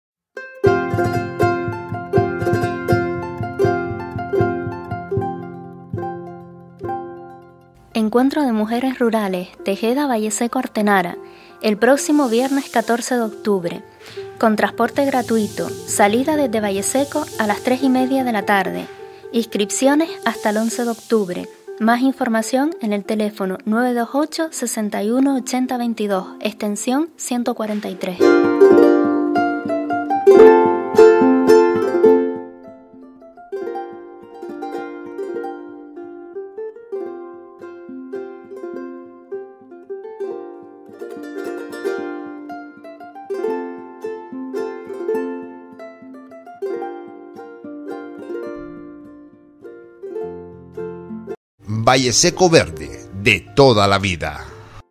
Encuentro de Mujeres Rurales, Tejada, Valleseco y Artenara
Viernes 14 de Octubre en el municipio de Artenara, con el objetivo de  conmemorar el Día Internacional de Las Mujeres Rurales, que se celebra el 15 de Octubre.

Transporte gratuito
Salida desde el Ayuntamiento de Valleseco a las 15:30 horas